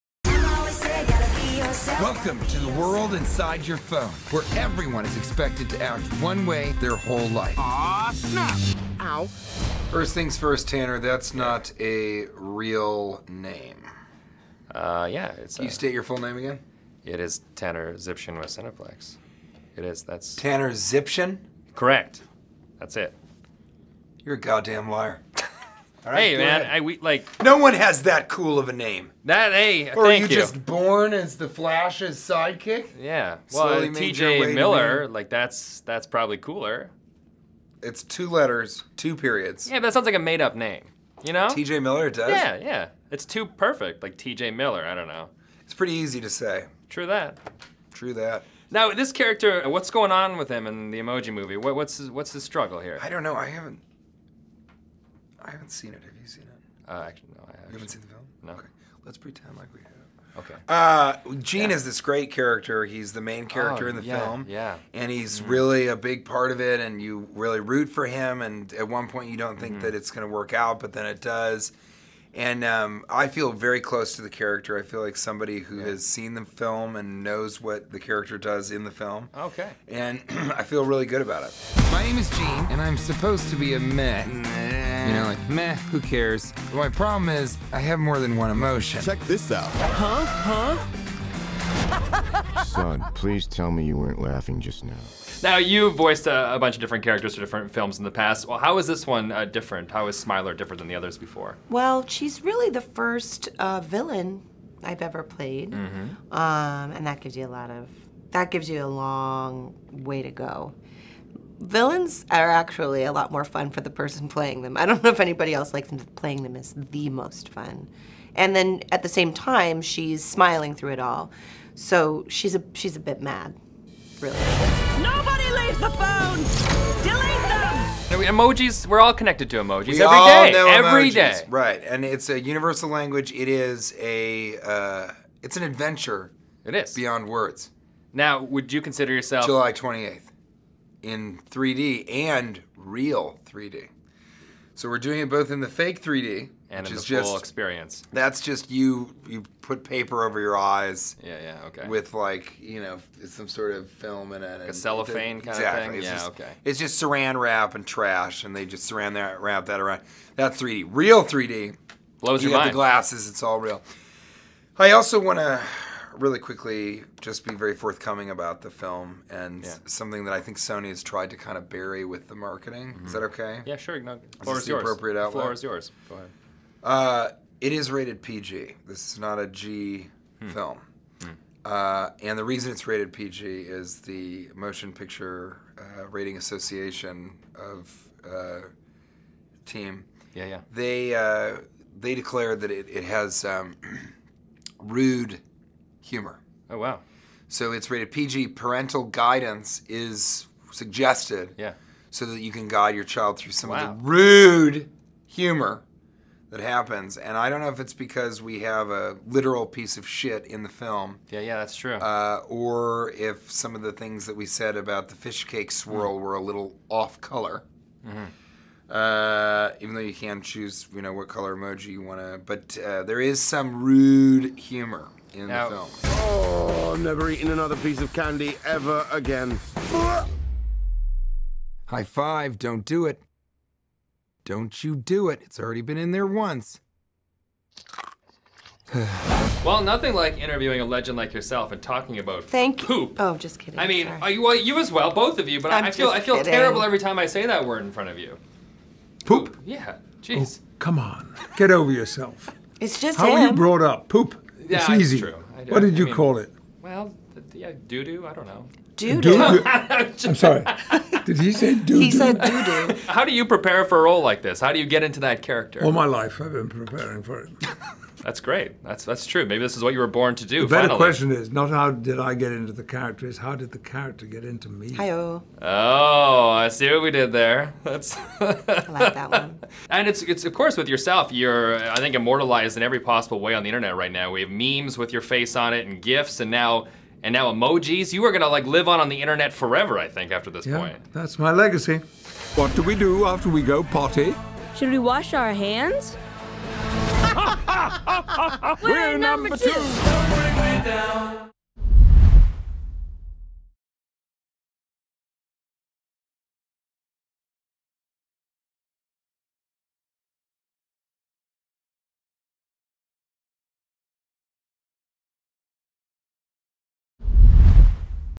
Tanner chats with Patrick Stewart, Maya Rudolph and T.J. Miller on The Emoji Movie
We talked to the cast of The Emoji Movie!
The Emoji Movie follows Gene, a multi-expressional emoji, who sets out on a journey to become a normal emoji.
THE EMOJI MOVIE opens at Cineplex theatres on July 28th. for tickets!